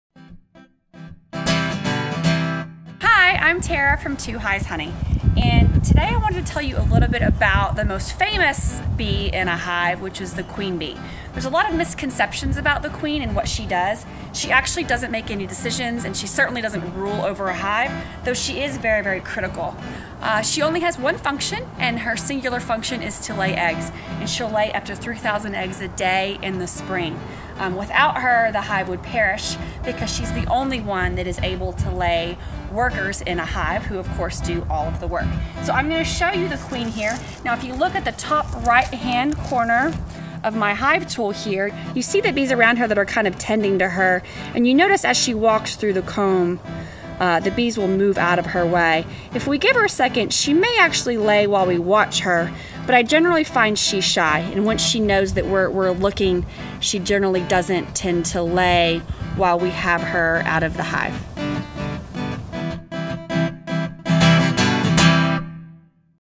Ask A Beekeeper - The Queen Bee
This week on "Ask a Beekeeper" . . . we're all about the Queen!